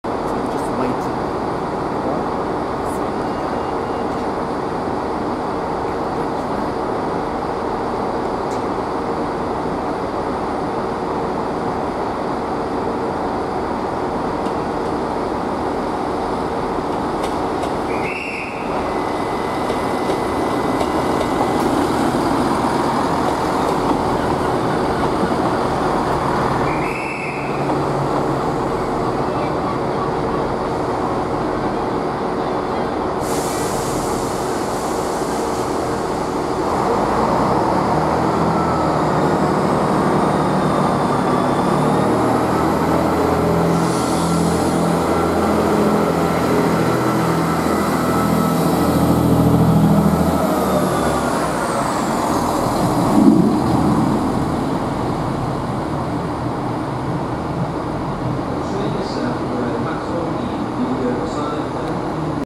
Arriva Trains Wales Class 158 Departing Cardiff Central (07/5/16)
2 Coach Services Arriving And Departing Cardiff Central.